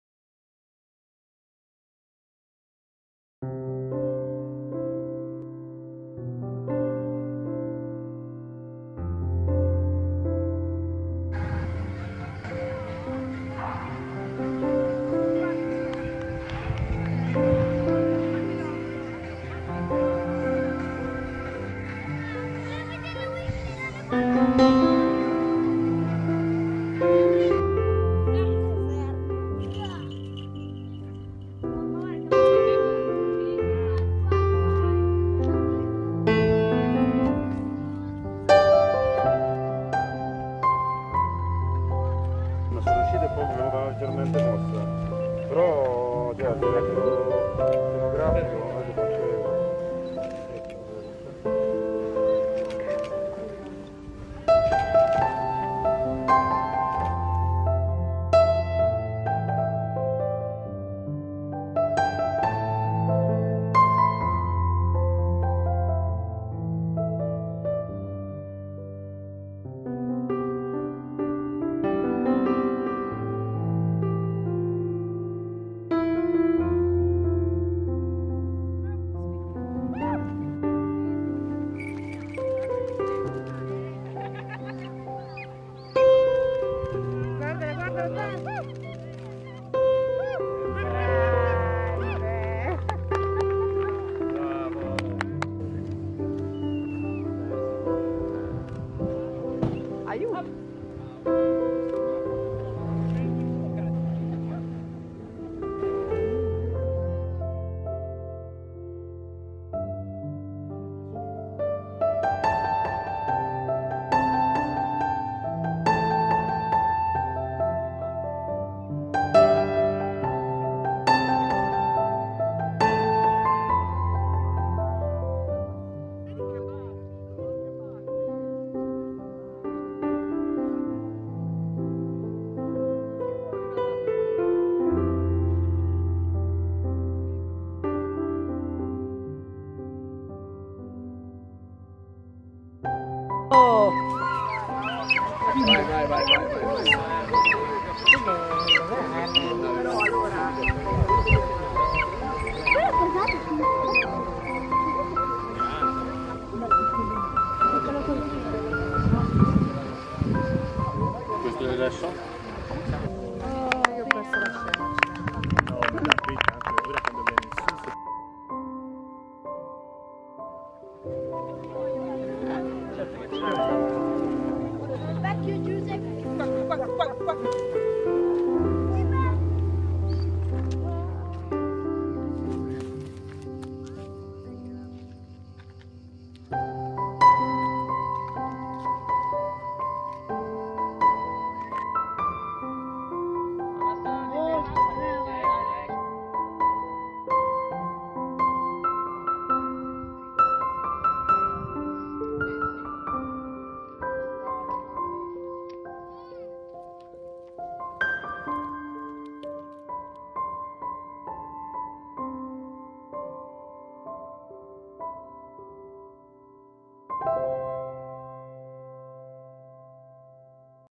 Raduno con le aquile a Monte Nerone
Secondo raduno dei falconieri con le loro aquile e non solo In cielo volavano anche il nibbio bruno, grifone ,l'avvoltoio, pappagalli Manifestazione veramente emozionante e con tantissima gente Ci rivediamo il prossimo anno